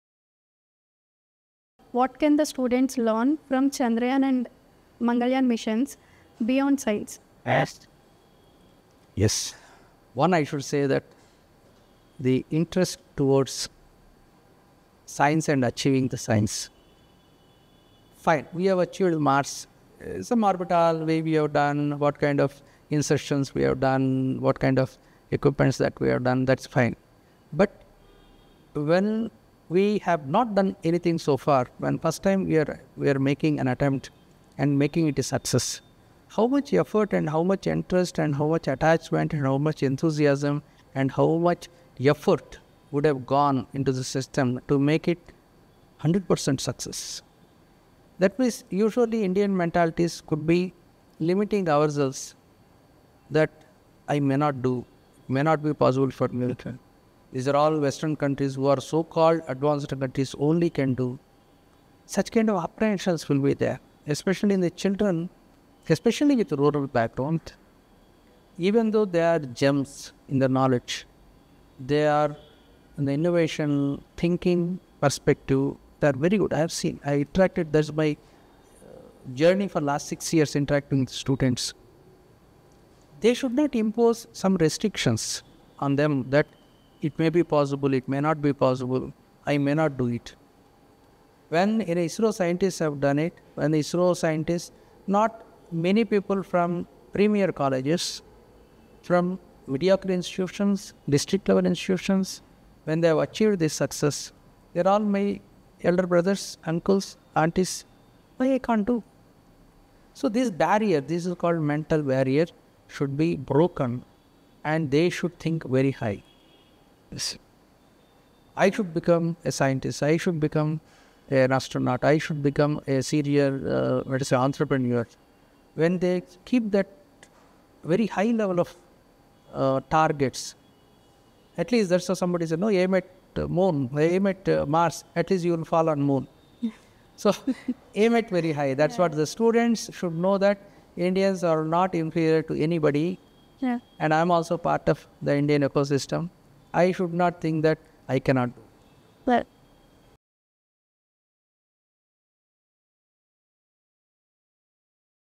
Swadeshi Silicon Podcast | Chandrayaan | Mangalyaan | Deep Tech | aerospace | key lessons
Chandrayaan and Mangalyaan show students how innovation, precision, and resilience can break boundaries no matter the budget or scale.